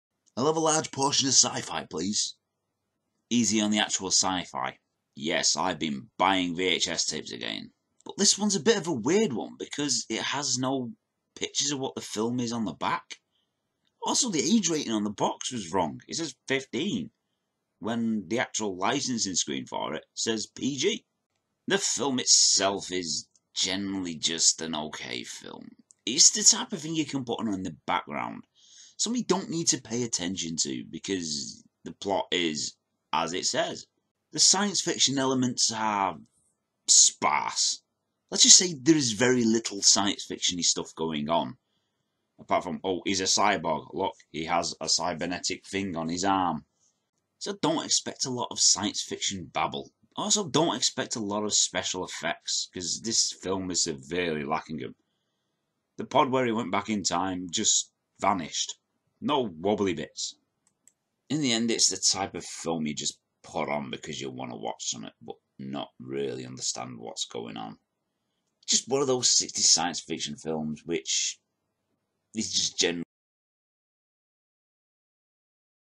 Cyborg 2087 - Film/Movie - One Minute review
Like a half budget pint of milk its another One minute reivew from me

This time of Cyborg 2087

A good background film, one that you can blank out to, one that you dont need to pay attention to